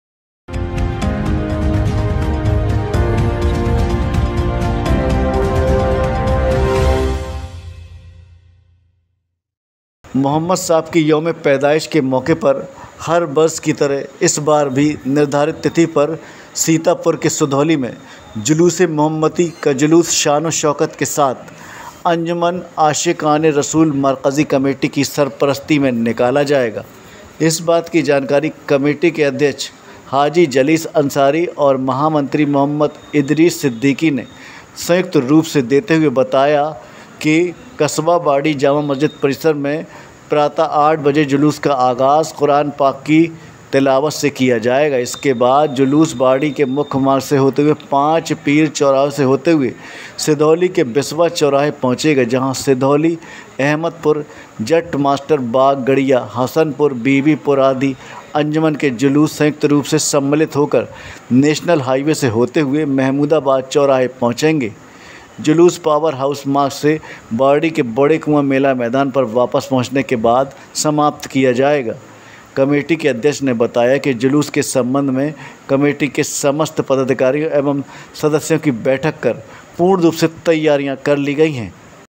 सीतापुर : सिधौली, जुलूस ए मोहम्मदी की तैयारियां पूर्ण, एक बैठक का आयोजन
सिधौली।। सीतापुर।। हजरत मोहम्मद सल्लल्लाहो अल० के यौम ए पैदाइश के मौके पर हर साल की तरह इस बार भी निर्धारित तिथि पर जुलूस ए मोहम्मदी का जुलूस शानो शौकत के साथ अंजुमन आशिक आने रसूल मरकज़ी कमेटी की सरपरस्ती में निकाला जाएगा इस बात की जानकारी कमेटी के अध्यक्ष हाजी जलीस अंसारी (प्रधान) एवं महामंत्री मोहम्मद इदरीस सिद्दीकी ने संयुक्त रूप से देते हुए बताया 10 अक्टूबर दिन सोमवार को कस्बा बाड़ी जामा मस्जिद परिसर से प्रातः 8:00 बजे जुलूस का आगाज़ कुरान पाक की तिलावत से किया जाएगा इसके बाद जुलूस बाड़ी के मुख्य मार्ग से होते हुए पांच पीर चौराहे से होते हुए सिधौली के बिसवा चौराहे पहुंचेगा जहां सिधौली अहमदपुर जट्ट मास्टर बाग गढ़िया हसनपुर बीबीपुर आदि अंजुमनों के जुलूस संयुक्त रूप से सम्मिलित होकर नेशनल हाईवे से होते हुए महमूदाबाद चौराहे पहुंचेंगे जुलूस पावर हाउस मार्ग से बाड़ी के बड़े कुआं मेला मैदान पर वापस पहुंचने के बाद समापन होगा कमेटी के अध्यक्ष ने बताया जुलूस के संबंध में कमेटी के समस्त पदाधिकारियों एवं सदस्यों की बैठक कर पूर्ण रूप से तैयारियां की गई हैं और लोगों से सद्भावना एवं भाईचारे के साथ जुलूस अदब व एहतराम के साथ निकाले जाने की अपील की गई है